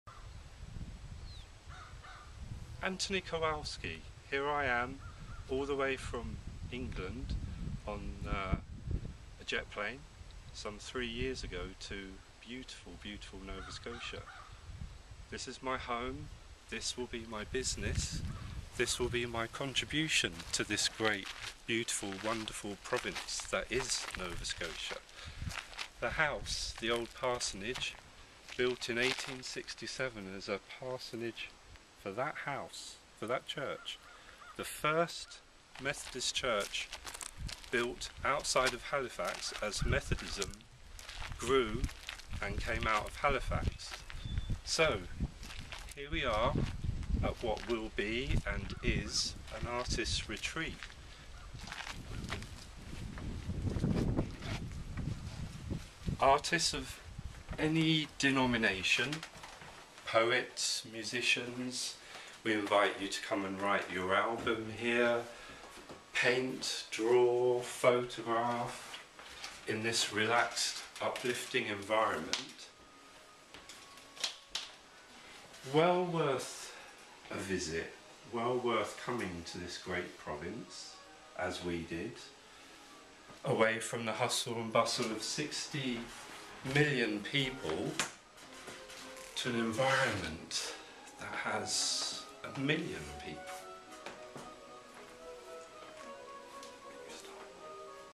The tale of The Old Parsonage, by Anthony Kowalski
Anthony Kowalski lives in Selma on a brooke, where he says Acadian-planted Barley still pops up in his backyard. This eloquent and kind-hearted Englishman has fallen in love with Nova Scotia, and he believes in developing the Maitland area as a rural and cultural hub. He thinks the best way to do it here, is to build something so shining it illuminates what we have and where we can go with it. He and his partner  started and run a uniquely designed artist & corporate retreat called The Old Parsonage out of an old church house.